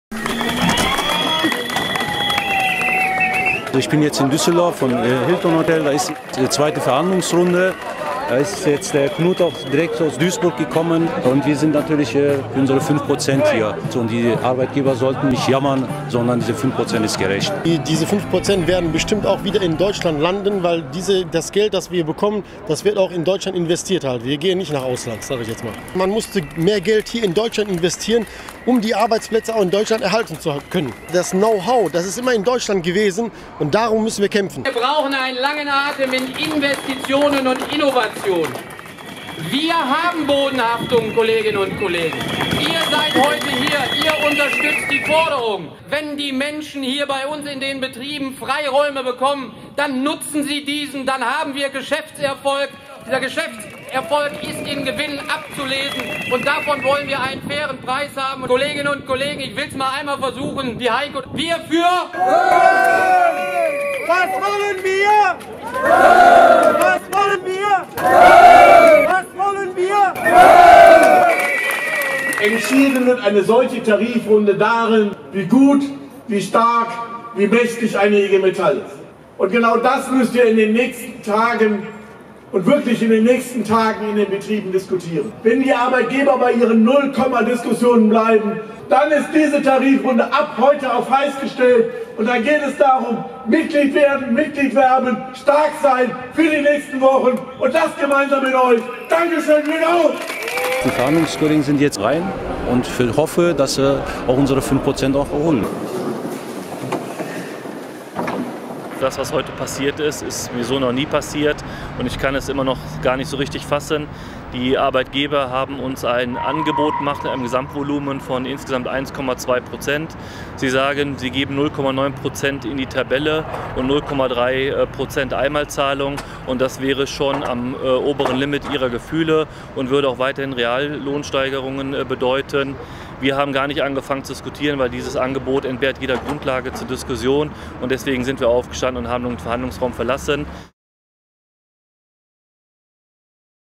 2. Verhandlung in der Tarifrunde Metall und Elektro 2016
400 Kolleginnen und Kollegen aus den Betrieben kamen am 11.04., um die Verhandlungskommission lautstark zu unterstützen. Trotzdem provozierten die Arbeitgeber mit einem beleidigend niedrigen Angebot.